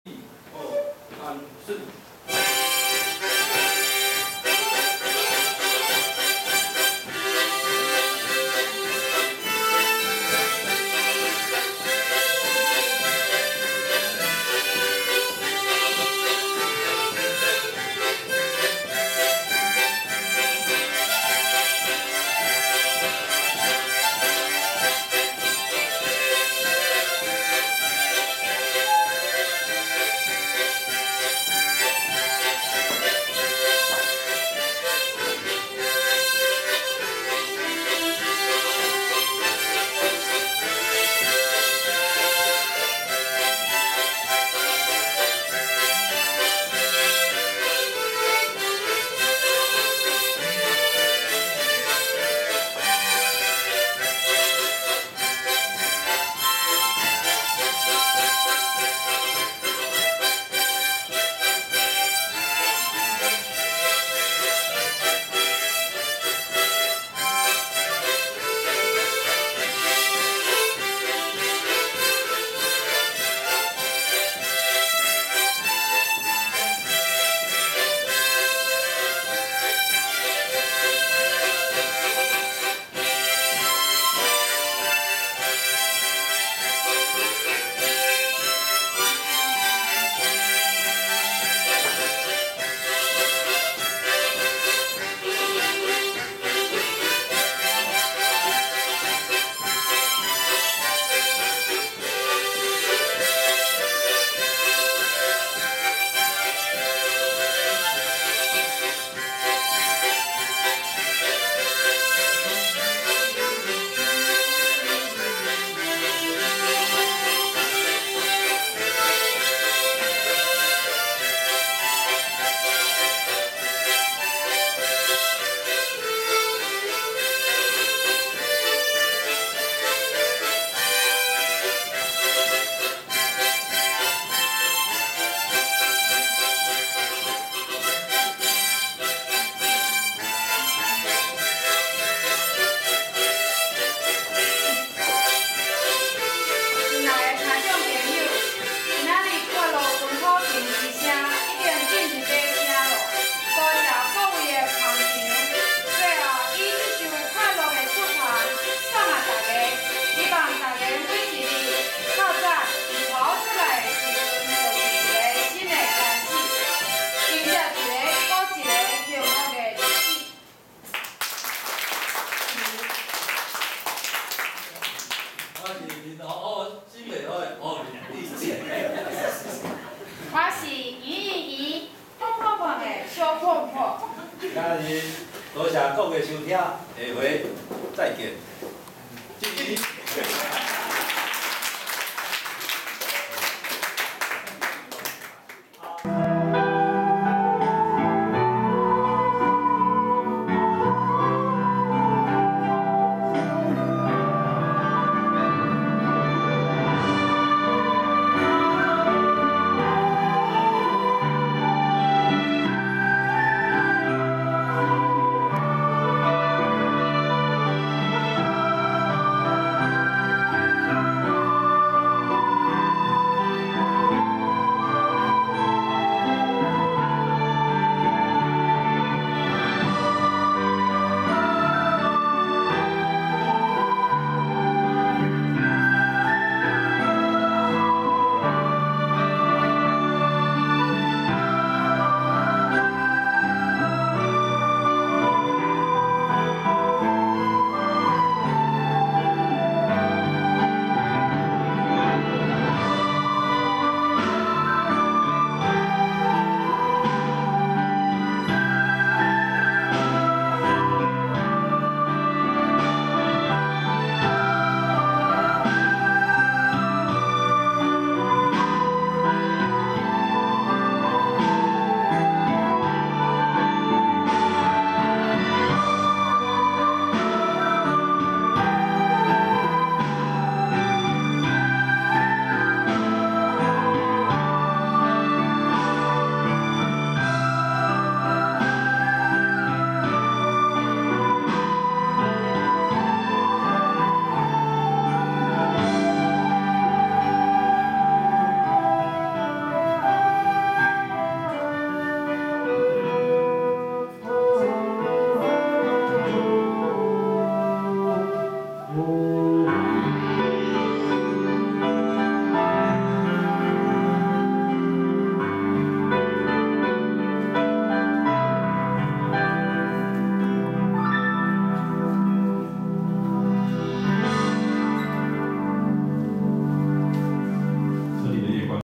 基隆社大─文化夜市2011 5 17.mpg
基隆社區大學為讓學員跨學程學習，於100年度春季班舉辦文化夜市活動，開放課程教學觀摩。